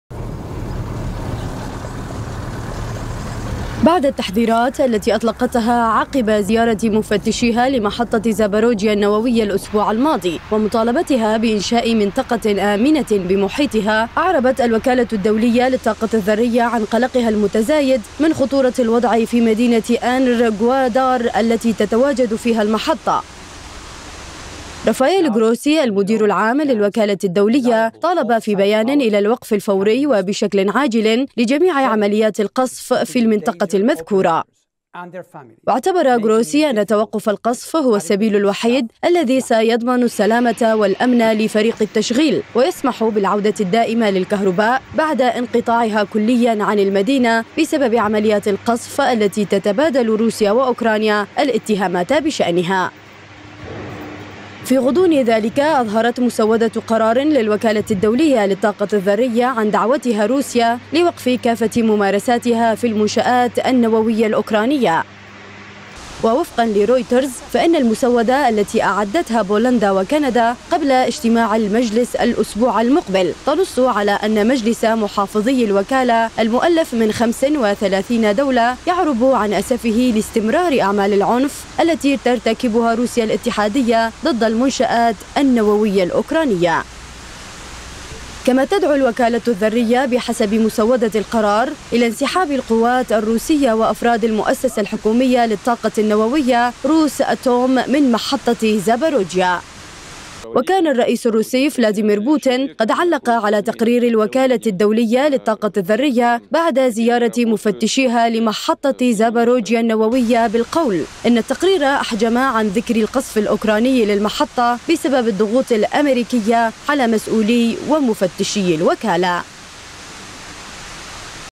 وكالة الطاقة الذرية تحذر من خطورة الوضع بمحطة زابوريجيا الأوكرانية
وكالة_الطاقة_الذرية تحذر من خطورة الوضع بـ
ــــــــــــــــــــــــــــــــــــــــــــــــــــــــــــــــــــــــــــــــــــــــــــــــــــــــــــــــــــــــــ
للمزيد من المواضيع يرجى زيارة منصات التواصل الاجتماعي لقناة اليوم:

تابعوا قناة اليوم على التردد:
 12687_27500 (H) _NILESAT